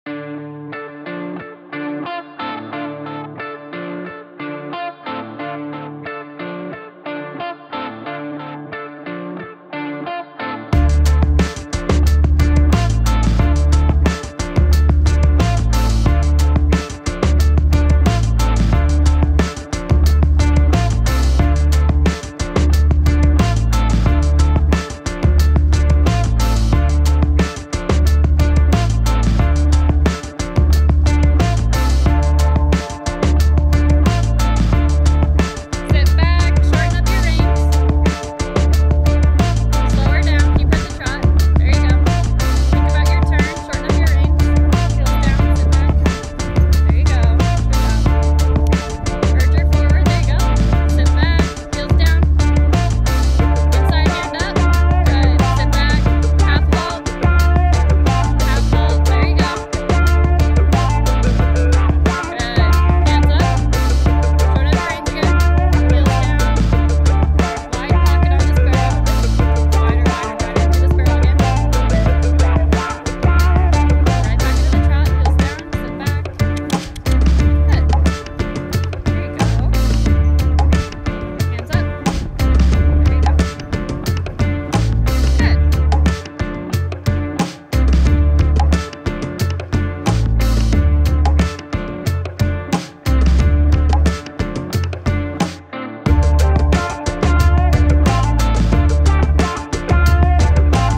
Training and selling baby ignore my old riding I was learning at that time
This one is about my horse baby I trainer her by myself and then got lessons on her